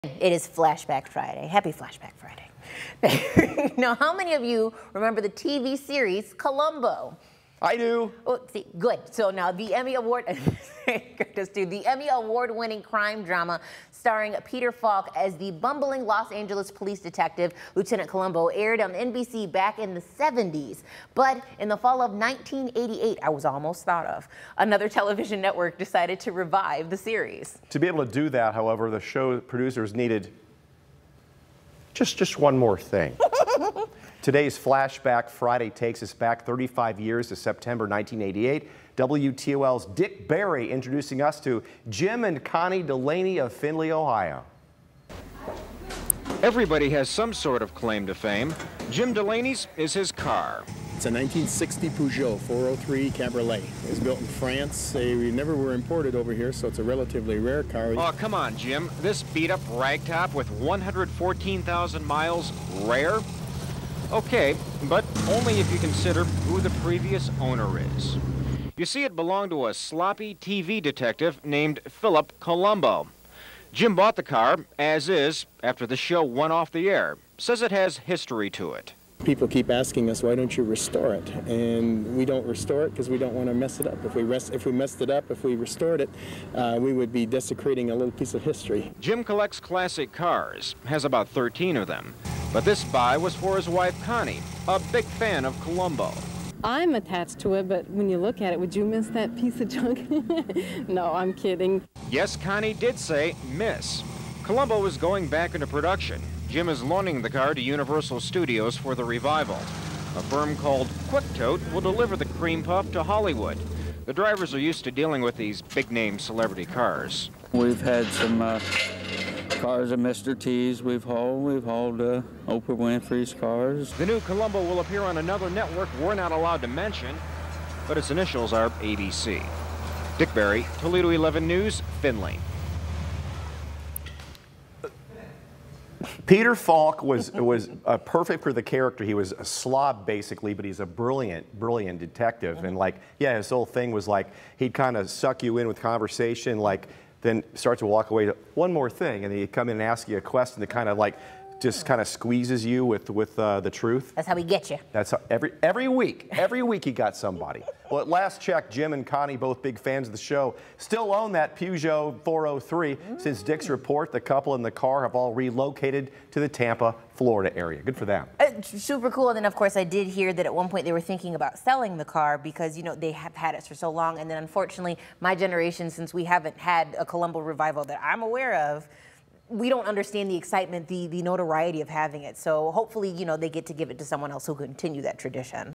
Flashback Friday: Do you remember "Columbo"? A Findlay couple was part of the show's 1988 revival
We're going back to September of 1988 when WTOL 11's Dick Berry introduced us to Jim and Connie Delaney of Findlay.